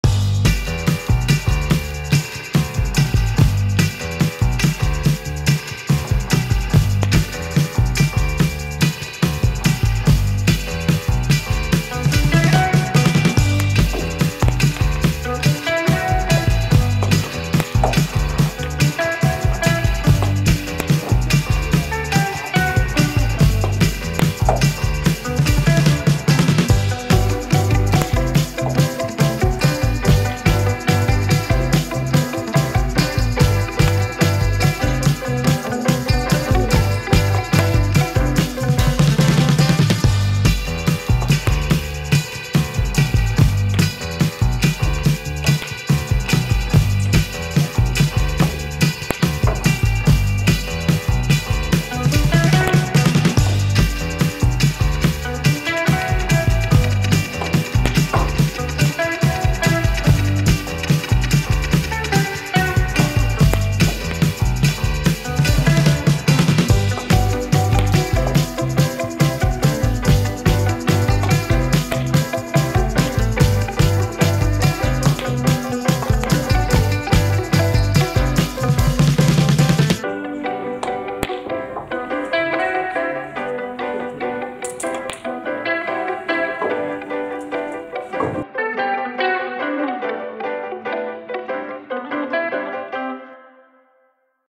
Steve Martin v Brian Wilson - Legends Tour Highlights
Brian Wilson took on Steve Martin for the first time since July, and the man known as the 'Ton' was searching for his 70th career victory over the opponent he began playing 17 years ago, but could he do it?